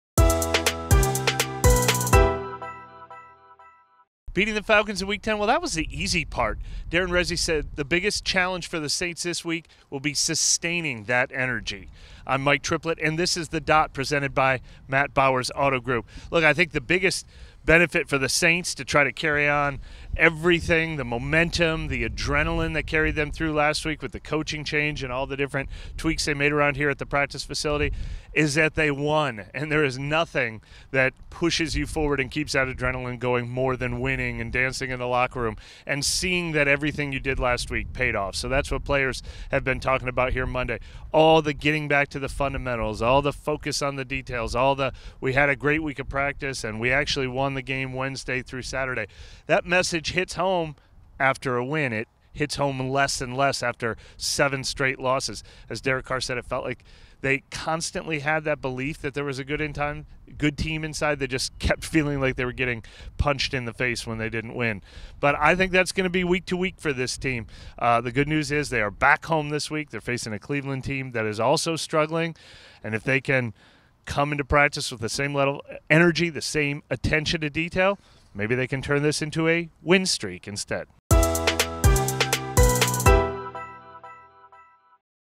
"Sustaining the energy" is Saints' biggest challenge now
Mike details how the Saints need to keep the energy high as they prepare for the Cleveland Browns

The Dot presented by Matt Bowers Auto Group